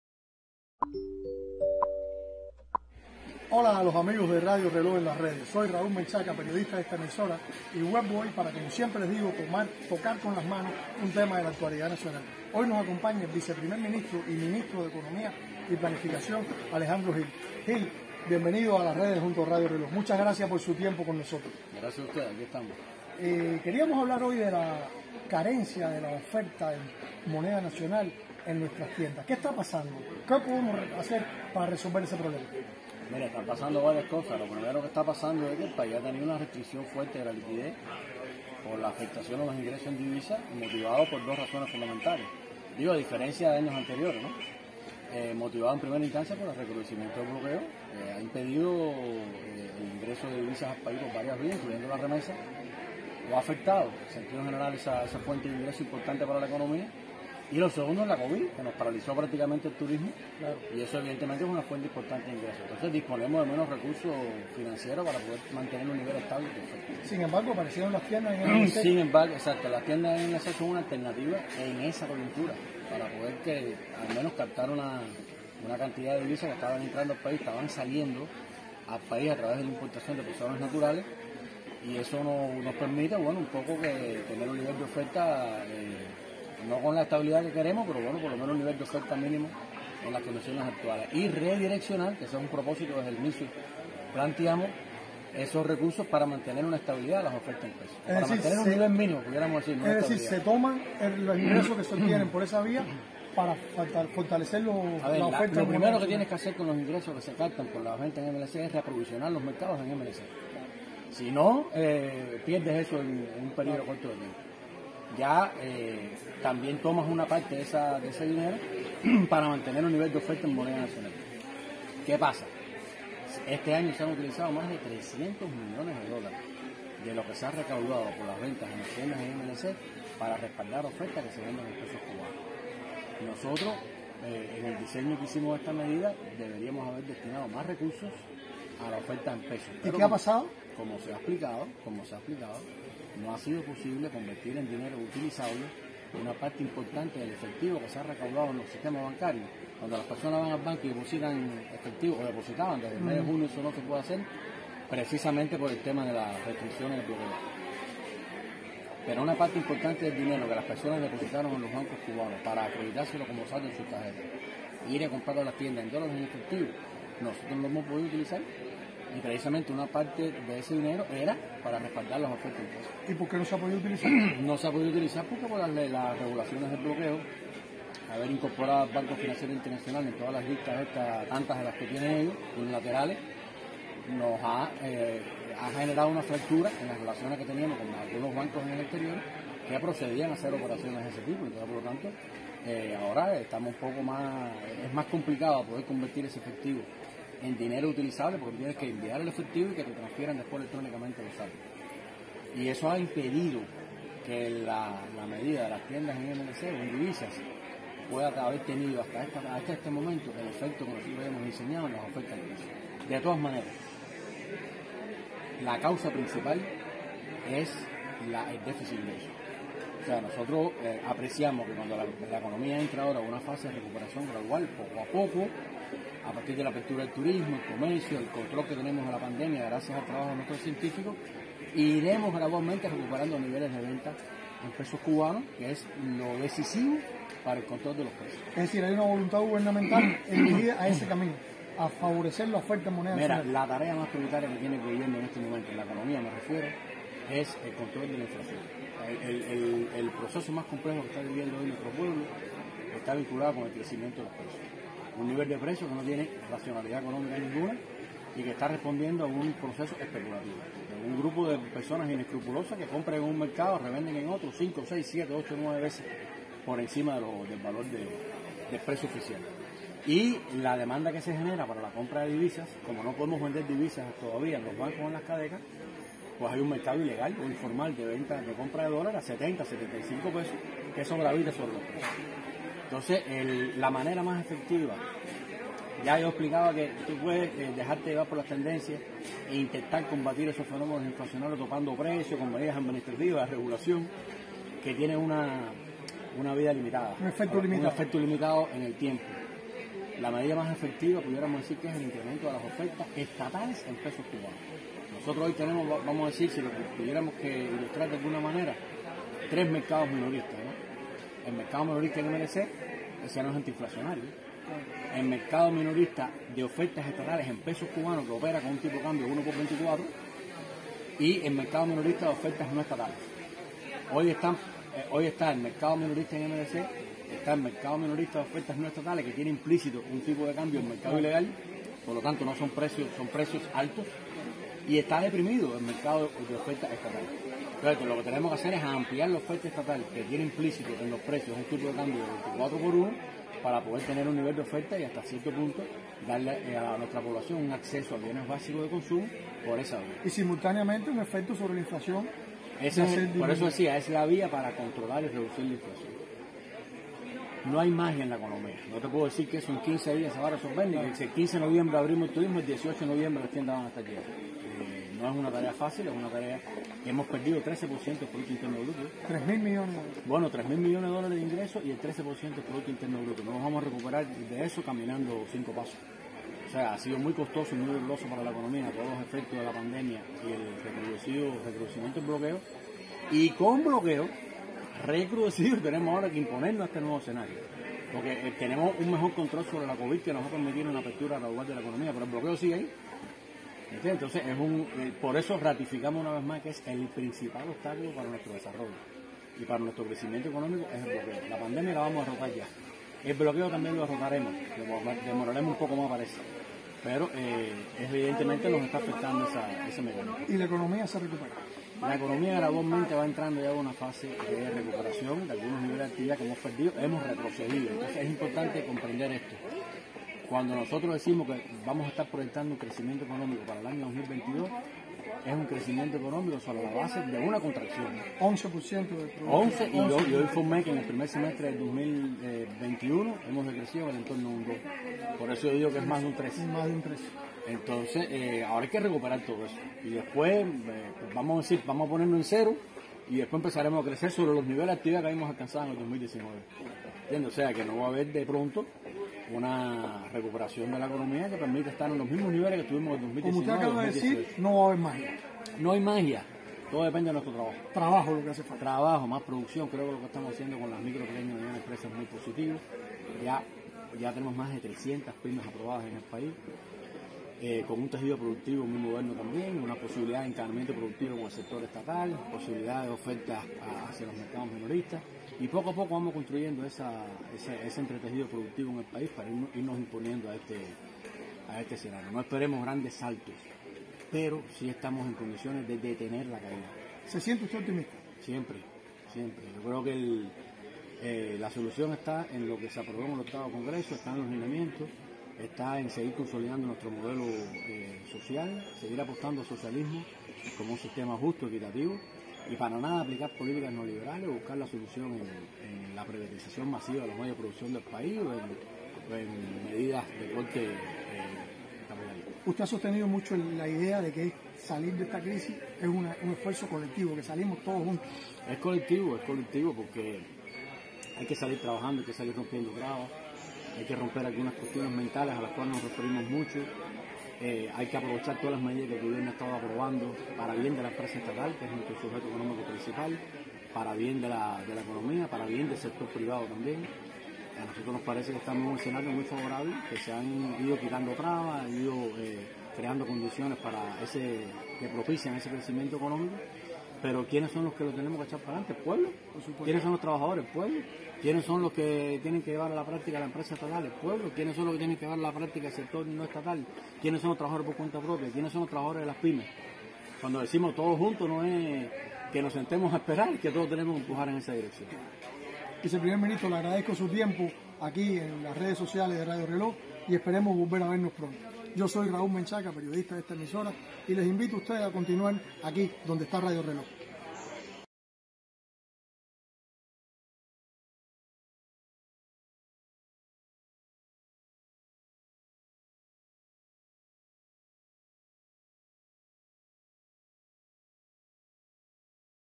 ¿Por qué hay poca oferta minorista en Cuba?
El periodista Raúl Menchaca conversa sobre la marcha de la economía con el vice primer ministro y titular de Economía y Planificación, Alejandro Gil Fernández
Fuente: CUBAHORA

29/10/2021